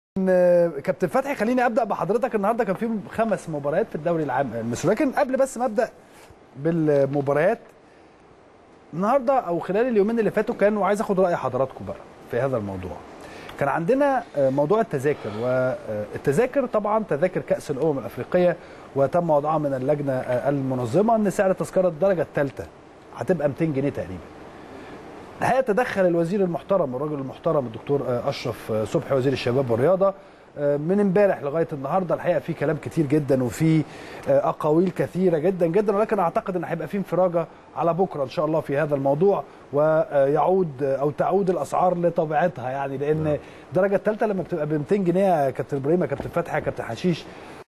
اسلام الشاطر : انفراجة قريبه فى اسعار تذاكر امم افريقيا بعد تدخل وزير الشباب والرياضة
التردد: 12341 أفقي.
القمر: نايل سات.
معامل الخطأ: 5/6.
معدل الترميز: 27500